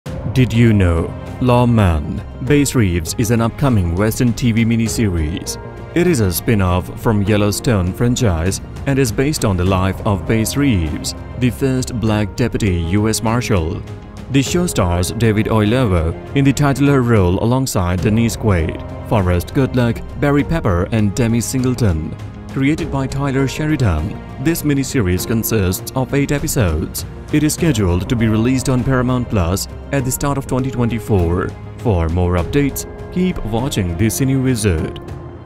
Lawmen Bass Reeves -TV Series
Did you know, Lawmen: Bass Reeves is an upcoming Western TV miniseries? It is a spin-off from “Yellowstone” franchise and is based on the life of Bass Reeves, the first Black deputy US Marshall. The show stars David Oyelowo in the titular role alongside Dennis Quaid, Forrest Goodluck, Barry Pepper, and Demi Singleton in the supporting roles. Created by Taylor Sheridan, this miniseries consists of 8 episodes. It is scheduled to be released on Paramount+ at the start of 2024. For more updates, keep watching The Cine Wizard.